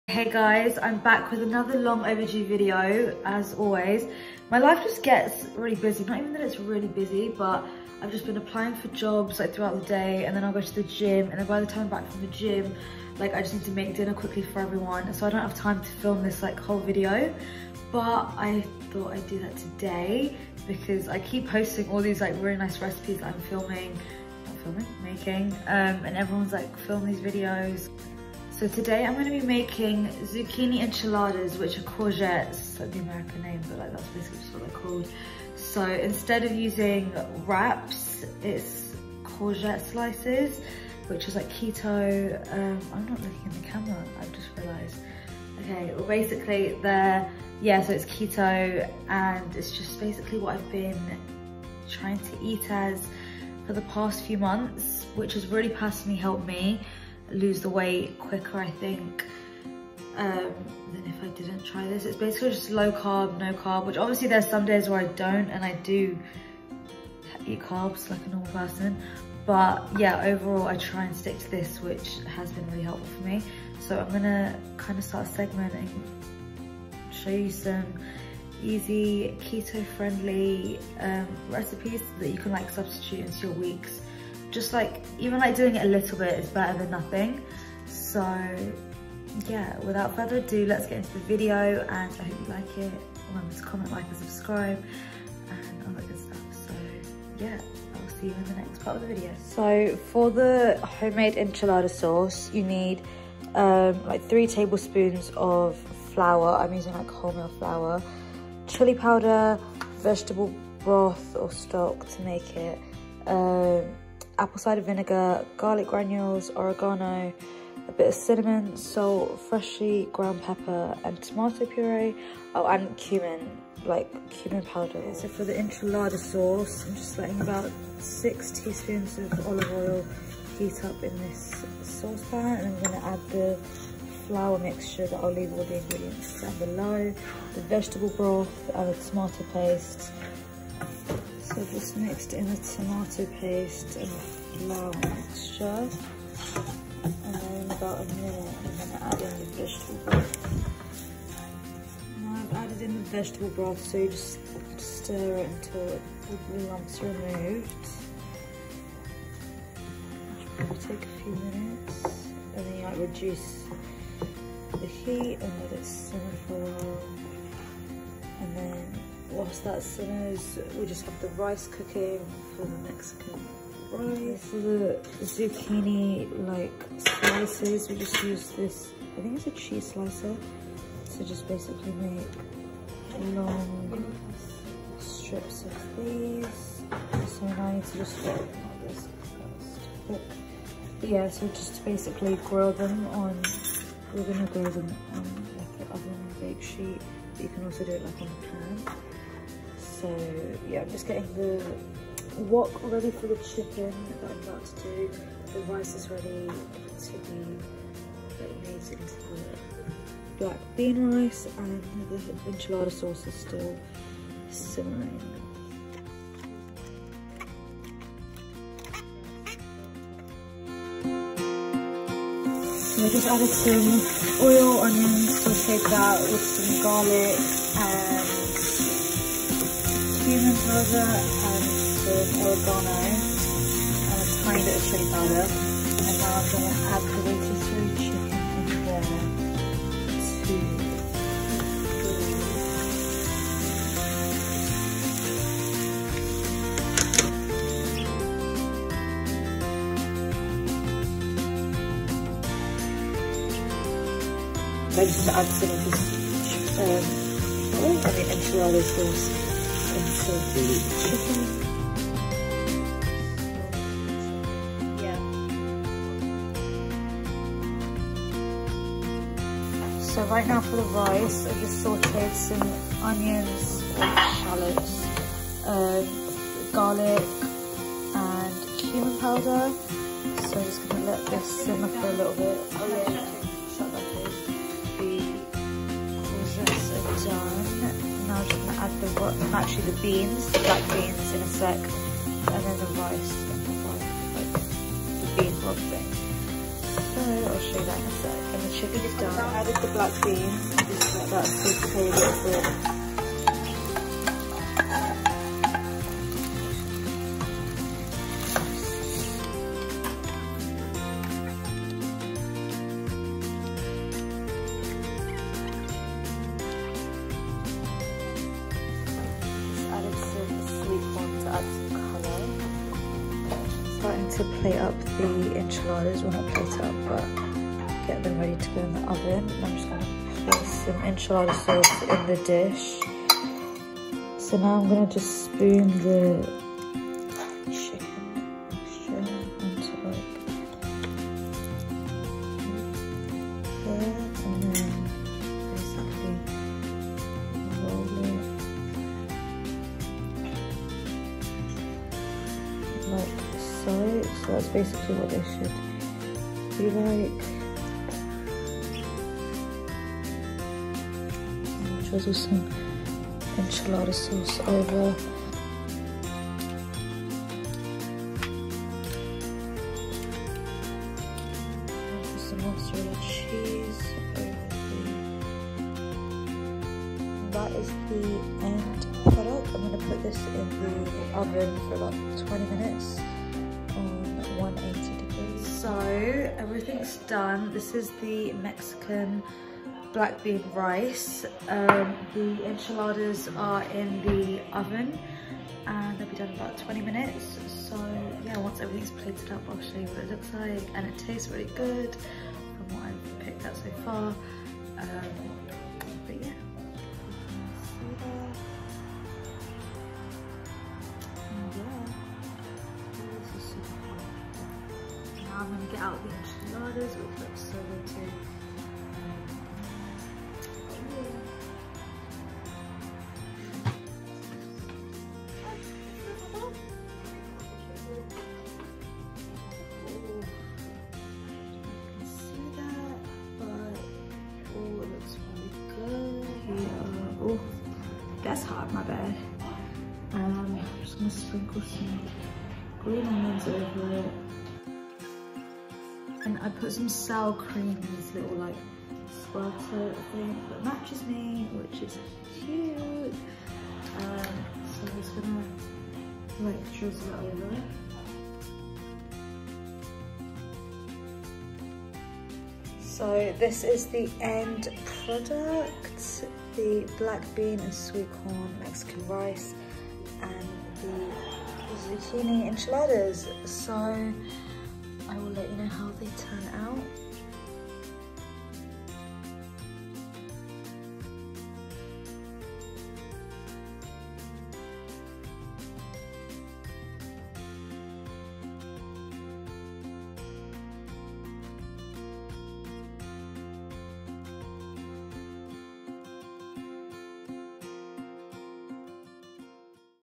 COOK WITH ASH: KETO ZUCCHINI / COURGETTE ENCHILADAS WITH BLACK BEAN RICE | ASHNI CHAUHAN
Hey guys, I hope you enjoy this keto recipe, I tried it and I loved it. Let me know if you'd like to see more videos like this. I'll write down all the ingredients below...

Serves 2/3 people:

Enchilada Sauce:
6 tablespoons olive oil
6 tablespoons flour (whole wheat flour, all-purpose flour and gluten-free flour blends all work!)
2 tablespoon ground chilli powder (scale back if you’re sensitive to spice or using particularly spicy chilli powder)
2 teaspoon ground cumin
1 teaspoon garlic powder
1/2 teaspoon dried oregano
1/2 teaspoon salt, to taste
Pinch of cinnamon (optional but recommended)
4 tablespoons tomato paste
4 cups vegetable broth
2 teaspoon apple cider vinegar or distilled white vinegar

Zucchini/Courgette Enchiladas:
2 or 3 Courgettes
3 tbs Olive oil
1 large Onions
3 cloves Garlic
1 tbs Cumin
1 tsp Oregano
1 tbs Chilli powder
Cilantro (optional to add to the chicken, I hate it so i skipped that part lol)
Sour cream
2/3 spring onions
Enchilada Sauce

Black Bean Rice
1 tablespoon vegetable or olive oil
¼ cup white onion finely diced
1 small garlic clove minced
1 cup of cooked black beans
1/4 cup black bean broth
1 ½ cup of cooked rice
1 cup of sweetcorn (optional)
½ teaspoon Mexican oregano
A pinch of ground cumin optional

Serve with Sour Cream & Make a homemade guac /salsa with tortilla chips

Lots of Love, Ashni